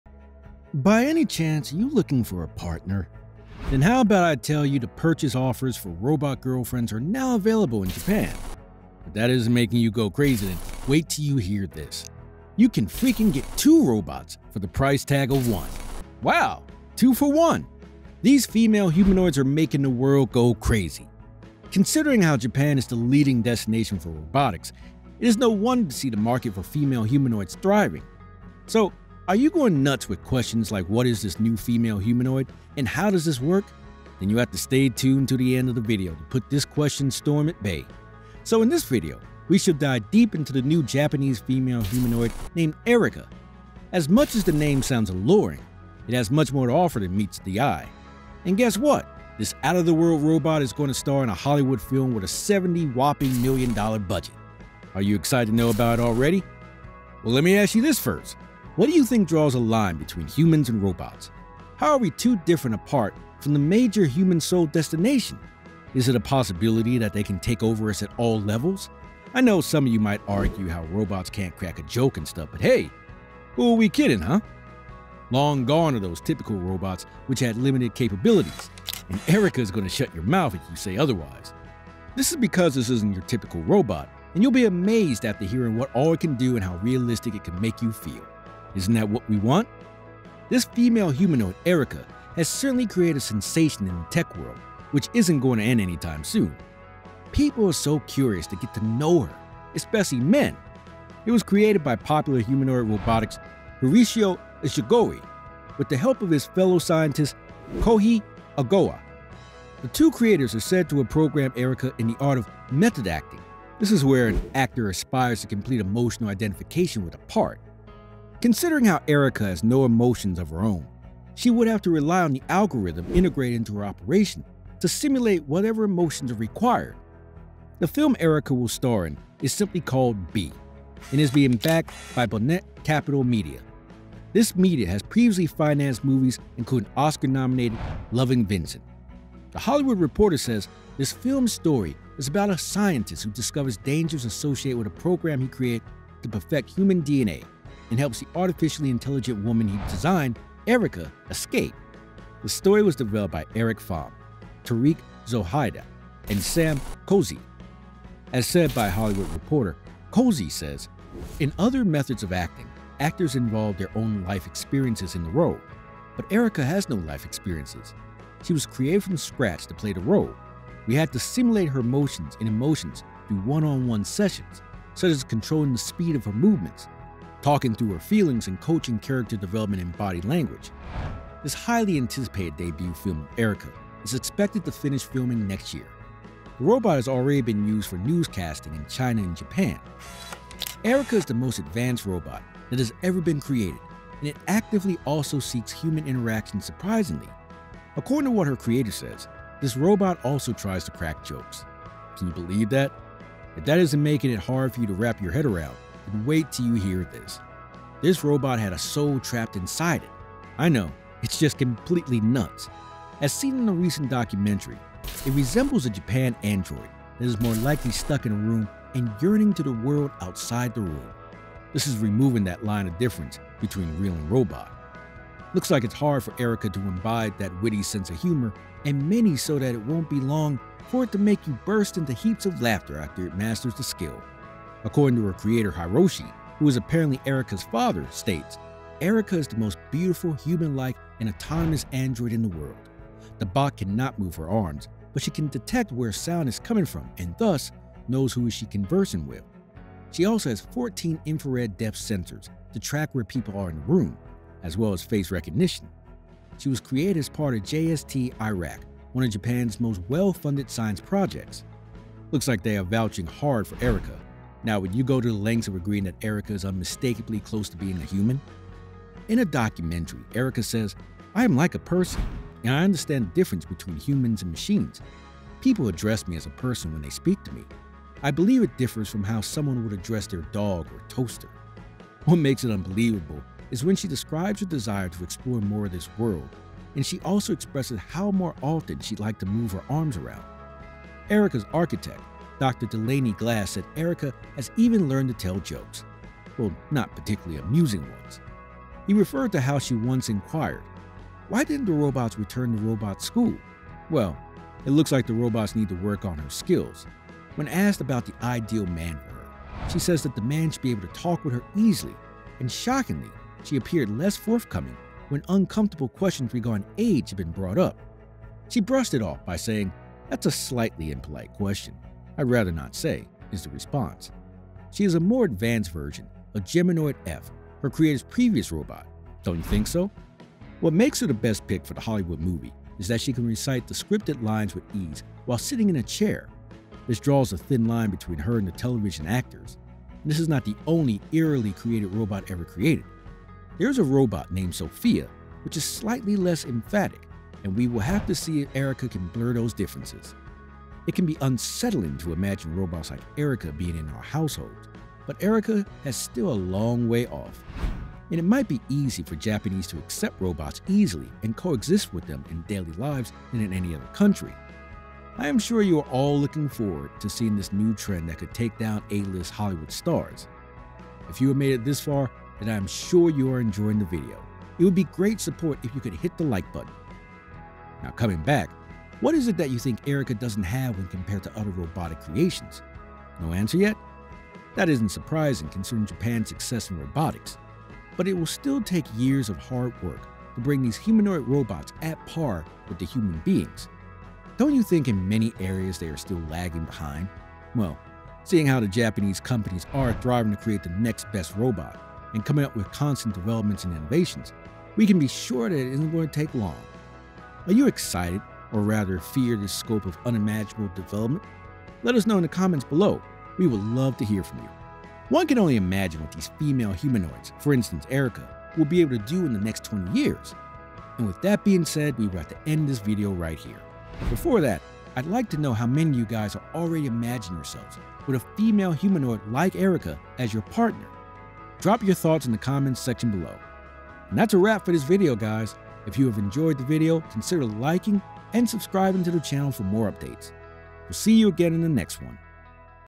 NEW Japanese Female Robots Released | Future Partner?
Japanese Female Robot

By any chance are you looking for a partner? Then, how about I tell you the purchase offers for robot girlfriends are now available in Japan. If that isn't making you go crazy then, wait till you hear this. You can freaking get two robots for the price tag of one. Wow! two for one. These female humanoids are making the world go crazy. Considering how Japan is the leading destination for robotics, it is no wonder to see the market for female humanoids thriving.